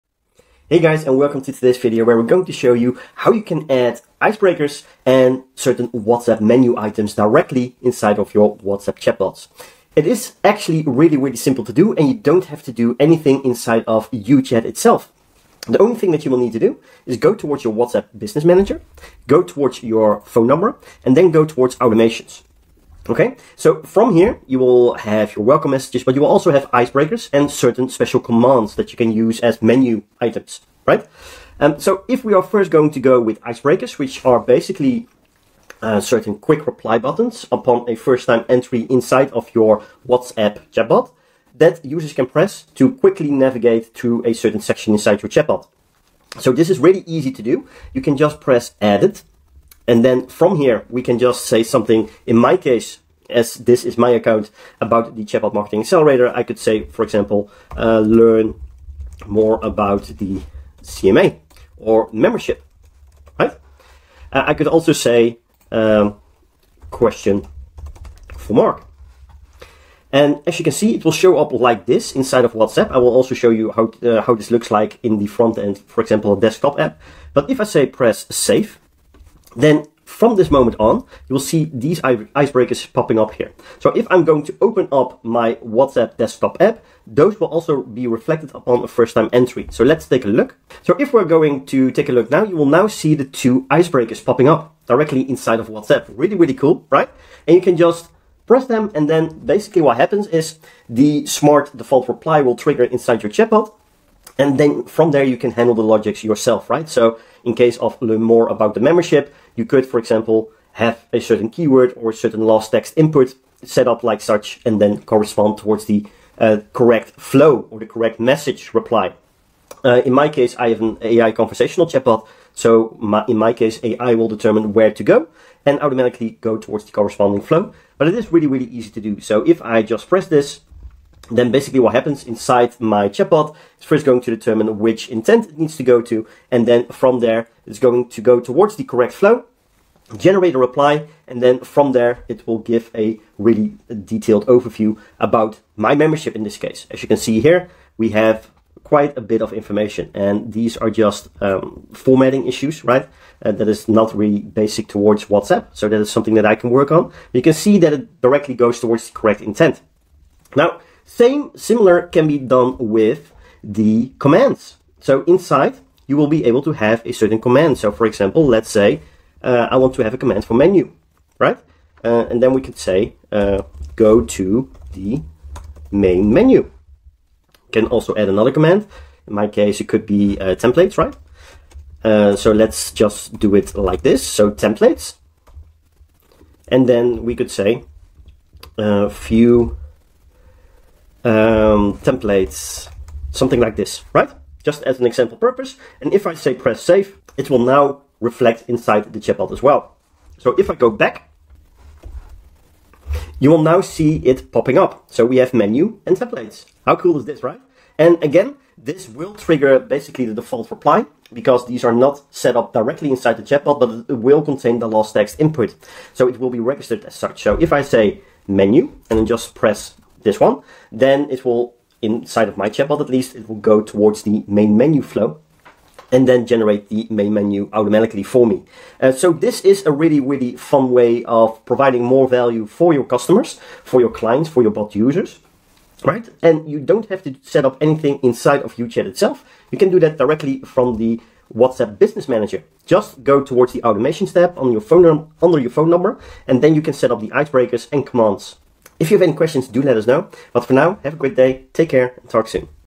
How to use icebreakers and commands in your WhatsApp chatbots
Today's video will show you how you can add more functionality to your WhatsApp chatbots by using icebreakers and commands